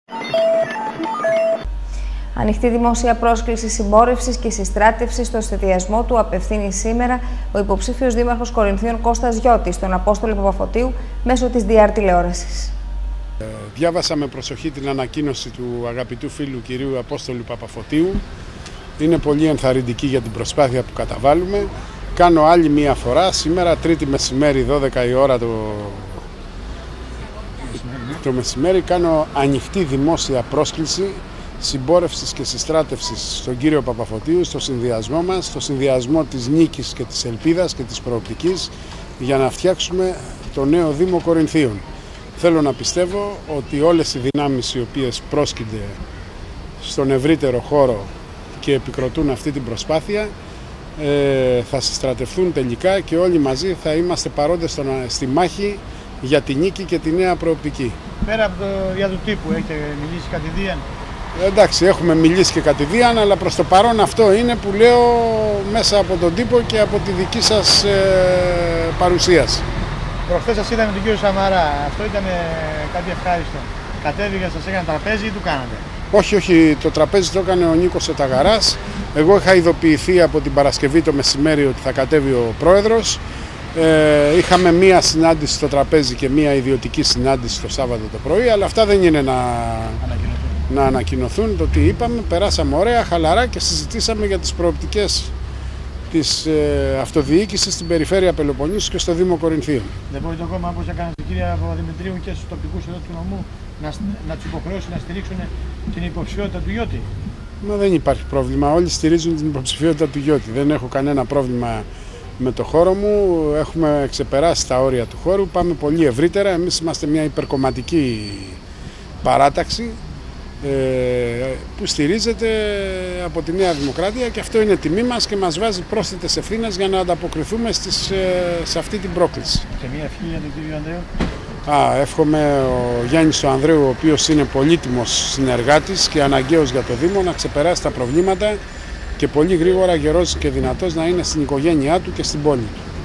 ΚΟΡΙΝΘΟΣ 1 9 2010 DRTV
Κ.Γιώτης - "Κάνω ανοιχτή δημόσια πρόσκληση συμπόρευσης & συστράτευσης στον Απ.Παπαφωτίου".